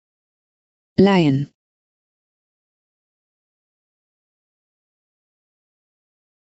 leihen
This is the Microsoft Hedda voice that is reading the word leihen